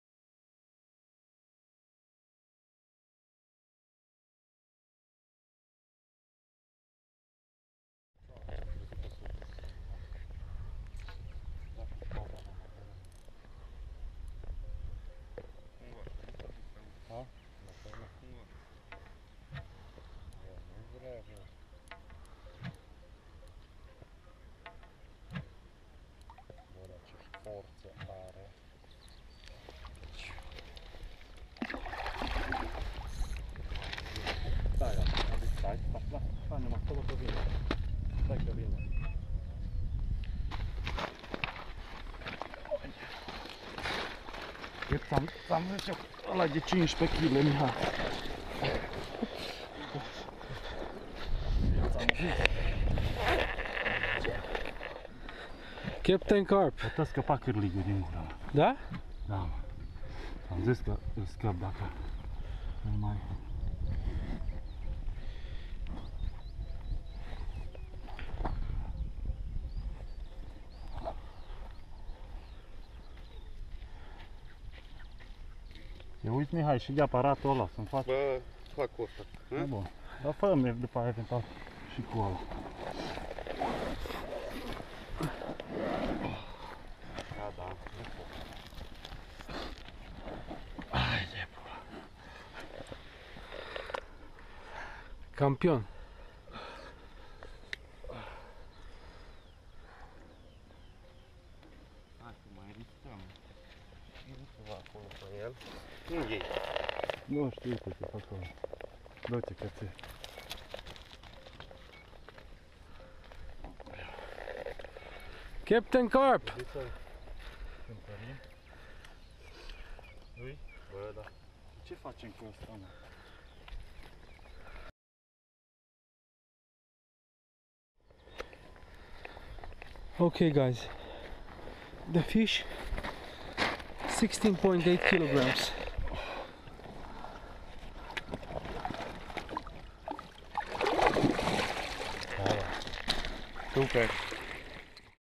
catching a 16.8Kg on Coas lake
Captain-carp's team catching on an inline rig the biggest catch on lake coax a 16.8 kg carp
a nice drill catch and release all on CC1+CC3